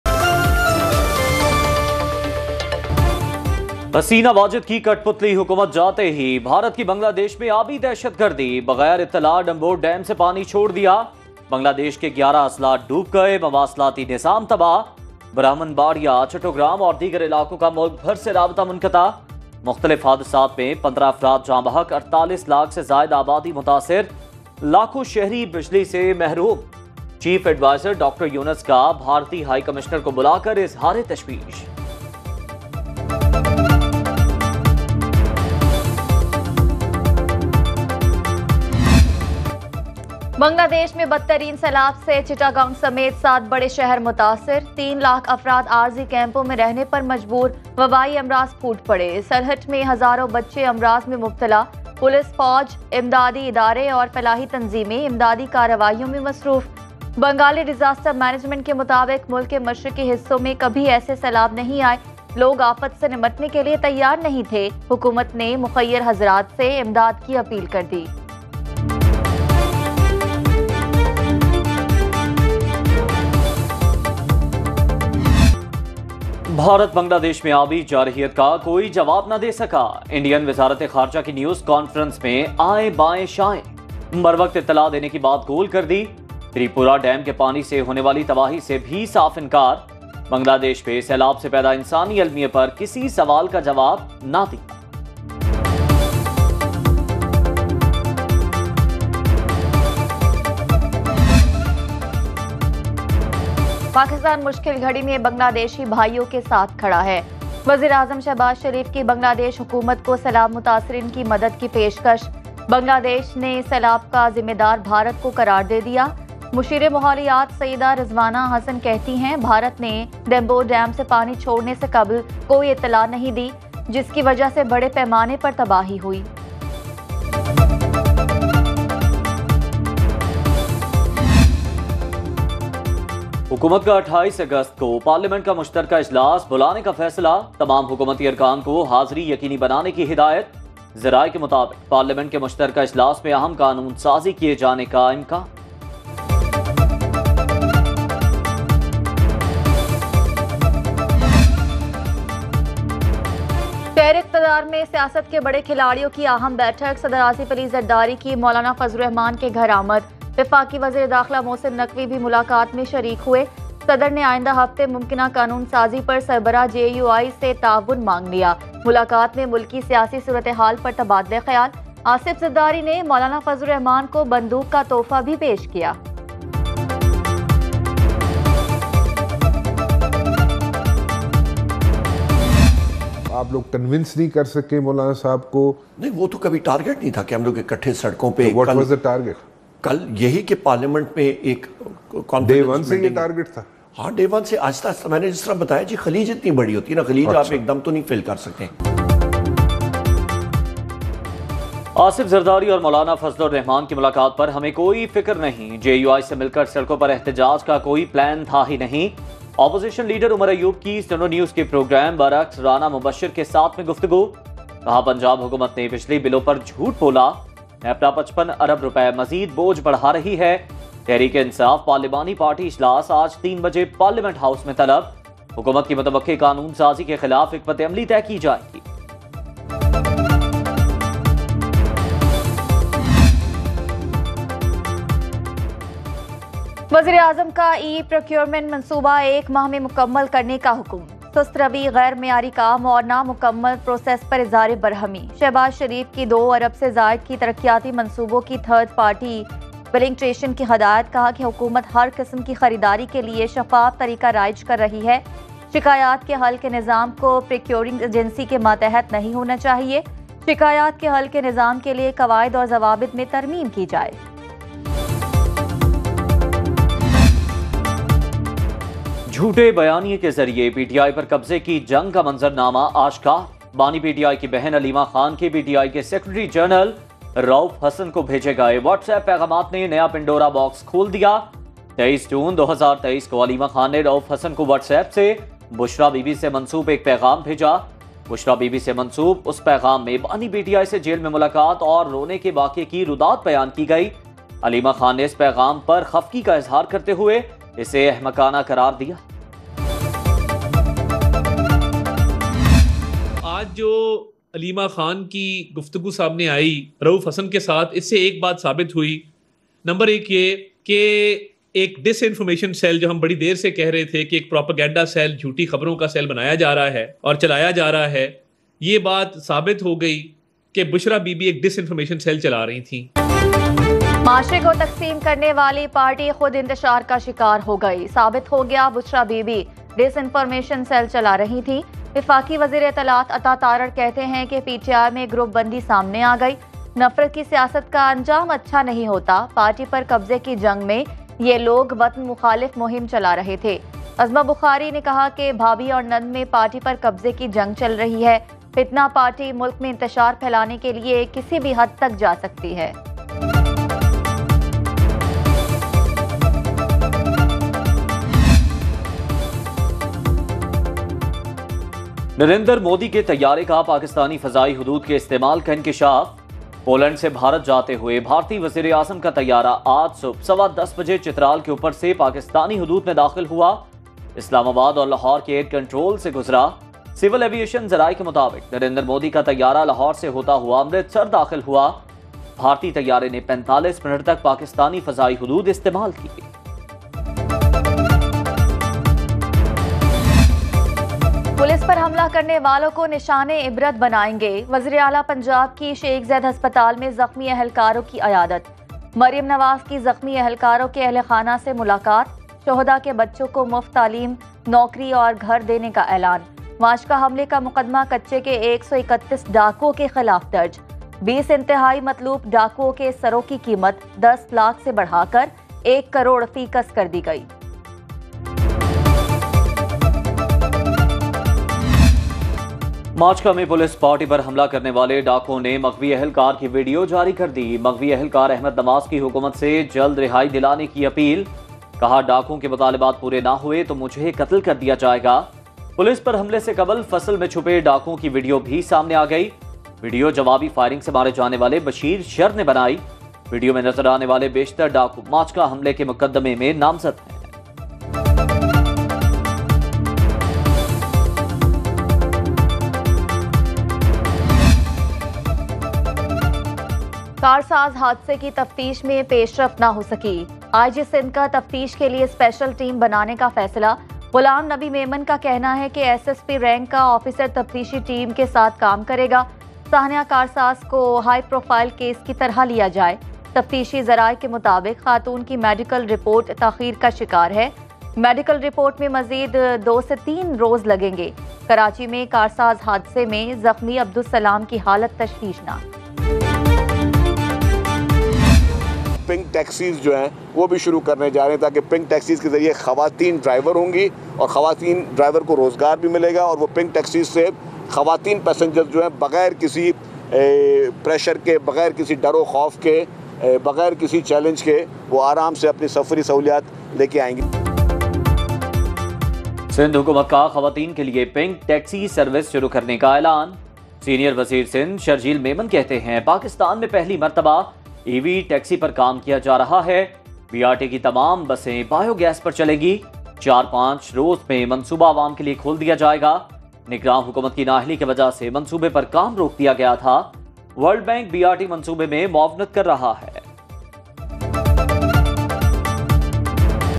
Bangladesh Floods Latest Updates | 12 AM News Headlines | 25 August 2024 | Suno News HD
"Stay updated with the latest headlines on Suno News HD! "Stay informed with the latest updates on the devastating floods currently affecting Bangladesh. In this video, we cover the extent of the flooding, the regions most impacted, and the ongoing rescue and relief efforts by the government and NGOs. Learn about the challenges faced by affected communities, the weather forecast, and the steps being taken to provide immediate aid and long-term recovery solutions.